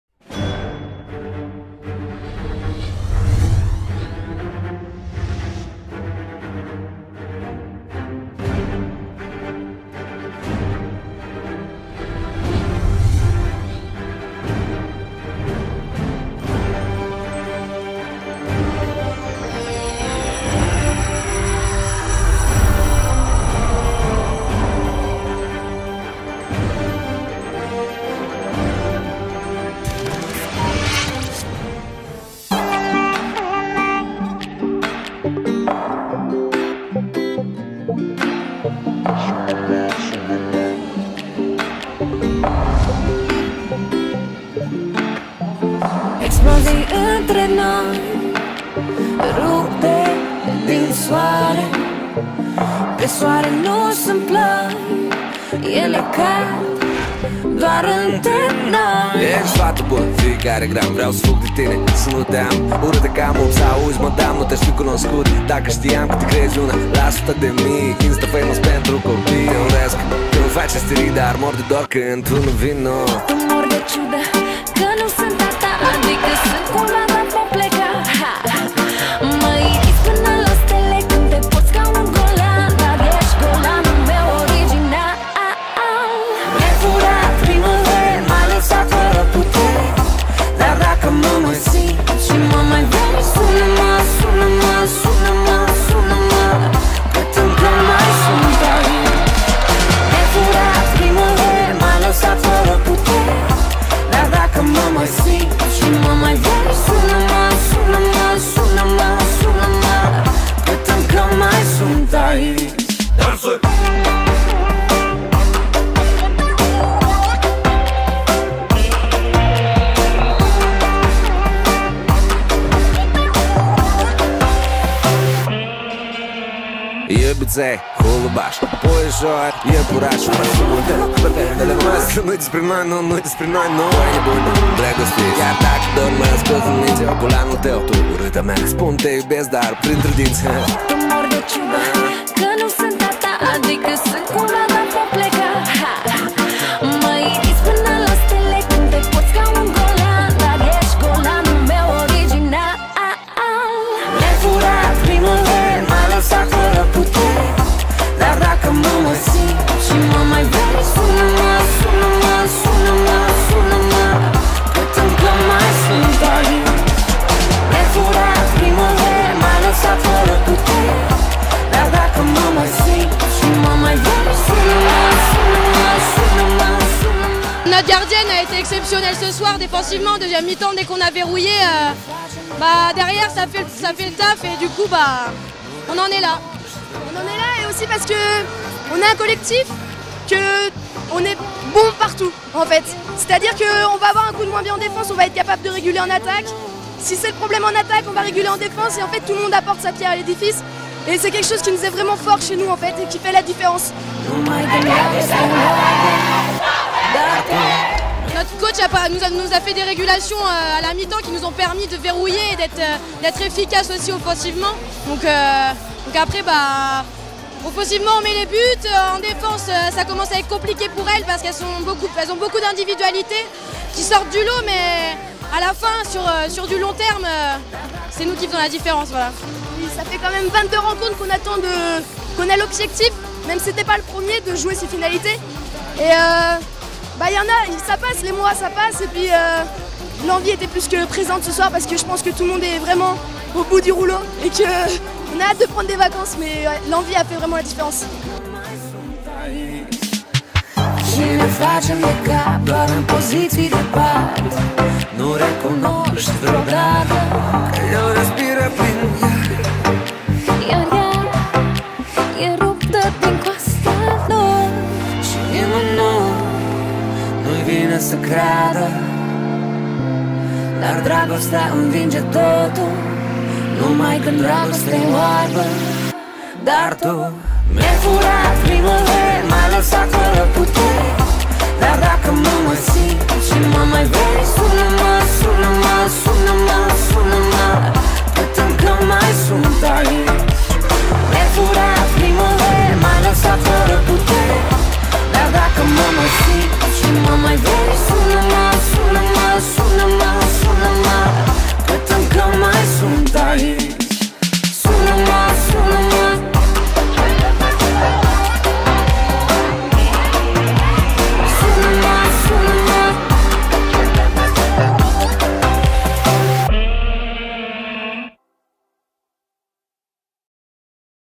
Finale Nationale 2 Féminine 2018 – L'Étoile vs Chambray THB 1B
Finalités des championnats de France de handball 2018
samedi 9 juin 2018
victoire du Chambray THB